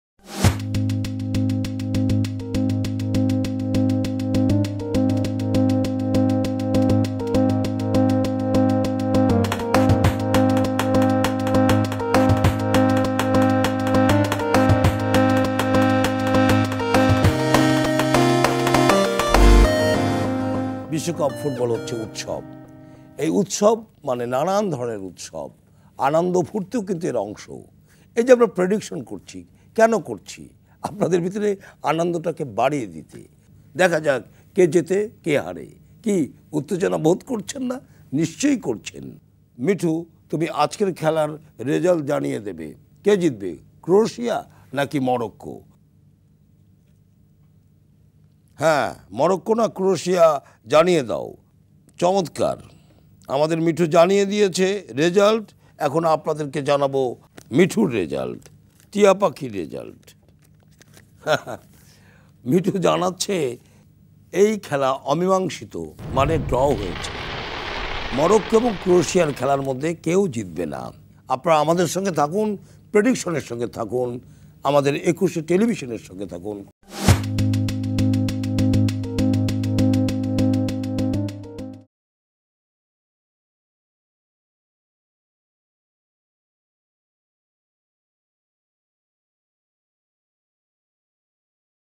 মরক্কো-ক্রোয়েশিয়া ম্যাচের ফলাফল নির্ধারণ করলো টিয়া পাখি! || Morocco vs Croatia || Ekushey TV
মরক্কো-ক্রোয়েশিয়া ম্যাচের ফলাফল নির্ধারণ করলো টিয়া পাখি!
মরক্কো-ক্রোয়েশিয়া ম্যাচে জিতবে কোন দল?